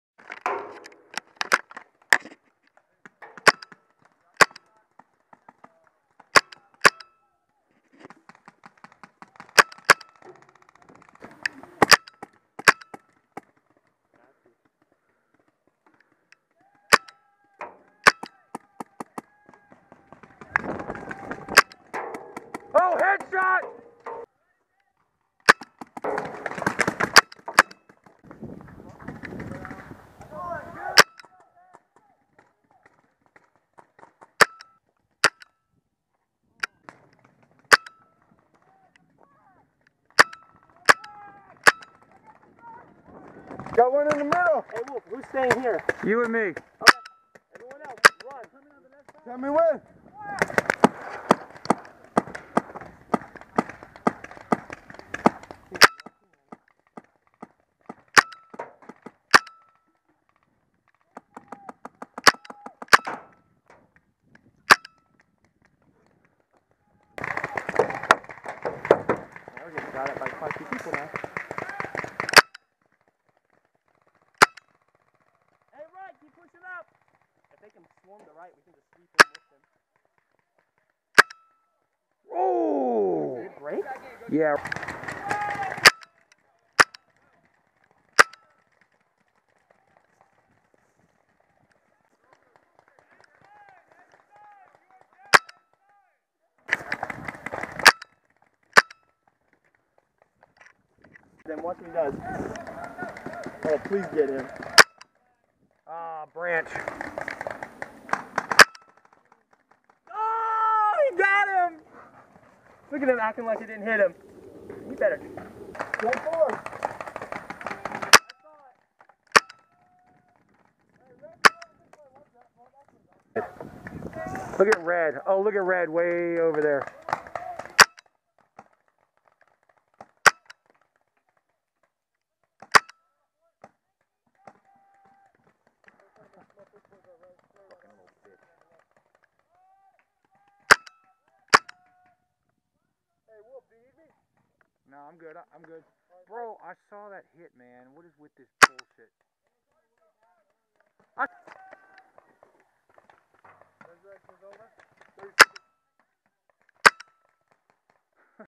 Scope cam Compilation
Trying out my new Scope cam set up to see how well it captures what's happening downrange.